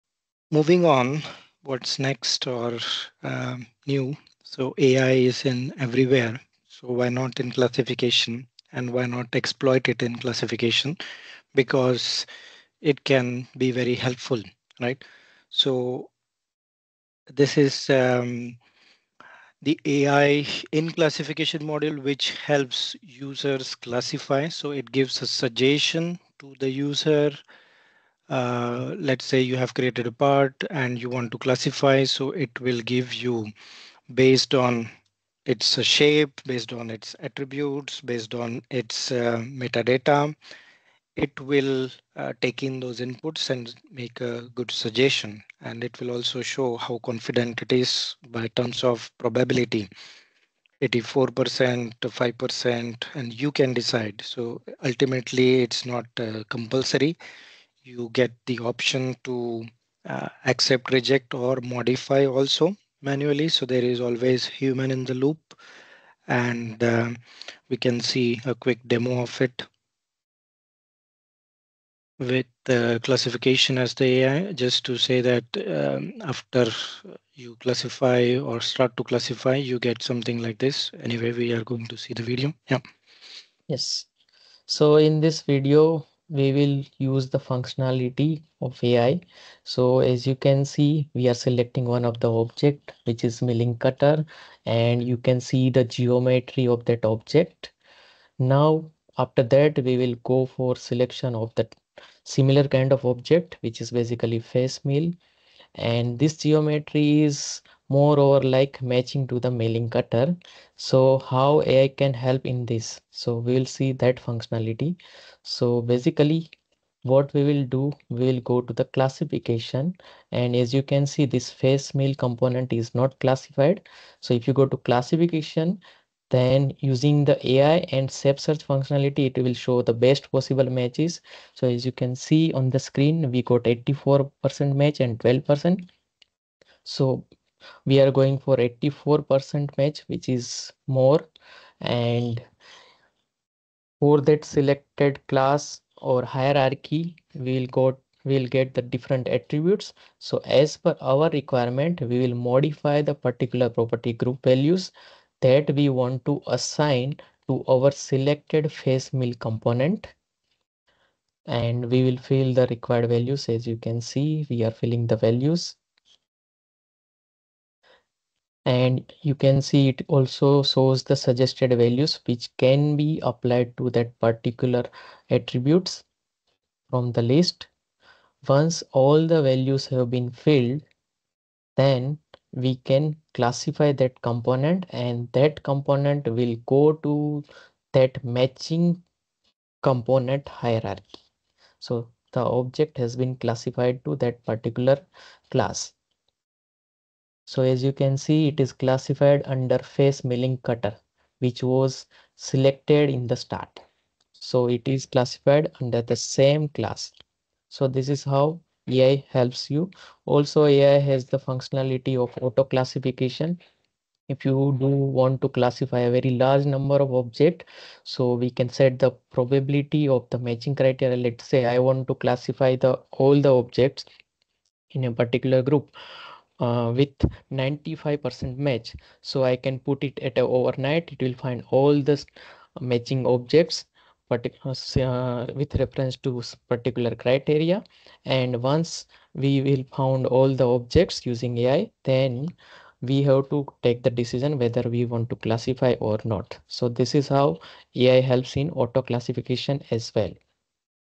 06_Teamcenter Classification with AI - PLM Nordic
Siemens has integrated AI and machine learning into Teamcenter’s classification module.

– Network between Teamcenter and classification data – An AI engine uses object classification in Teamcenter to create a neural network of classification knowledge.

– Prediction Accuracy – More components are classified; the neural network is gradually changed to improve prediction accuracy.

– Functional with Geolus Shape Search – The AI engine may integrate with Siemens’ Geolus shape search solution (optional; available if the ecosystem allows); for more accurate classification, a Closed Loop Weighted algorithm (patent pending) integrates meta-data and shape recommendations.

– Auto-classification – Auto-classification has built-in review processes that allow subject matter experts to confirm or modify classifications, and users can run them as background jobs.